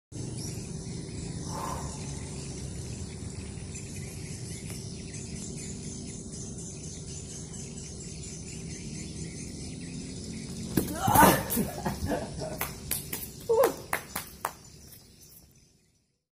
Full Planche Attempt - Lianto Salie
My Full Planche Attempt after Years of Practice Haha

Singkawang Calisthenics by Lianto Salie